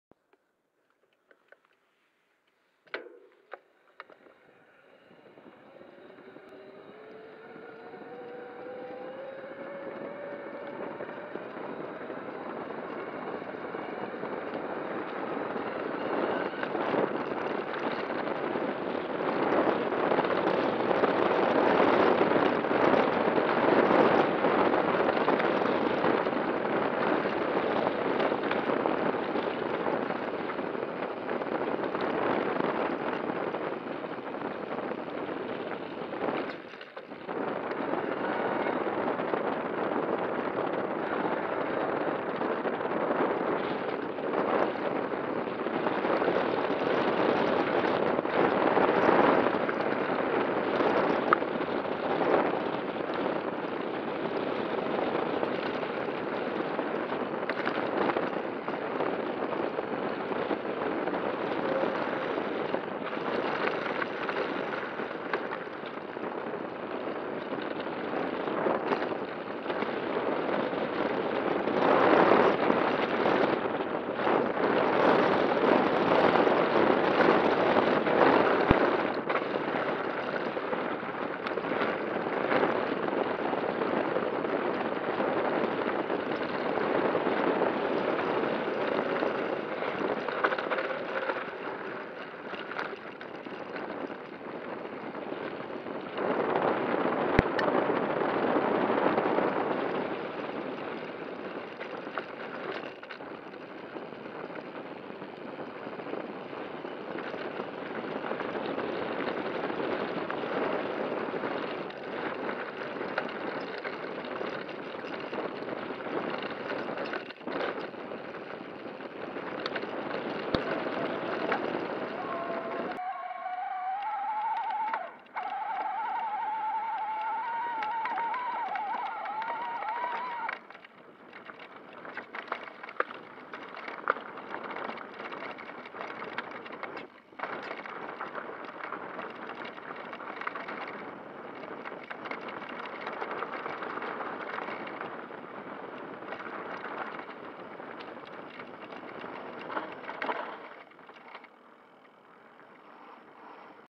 Downhill Enduro Ebike Viana do castelo
Round one
Camera: Runcam 2
Bike: Scott E genius 2019
Rider: Ghost Rider
POV: Front suspention Fox 36 / Rear shock Fox nude evol